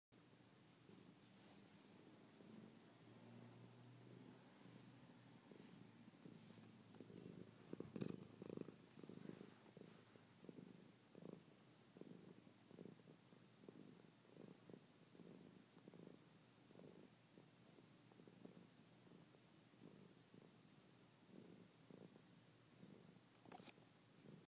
Maggie's Purr
Maggie has a great, loud purr--except when trying to record it. Good capture midway.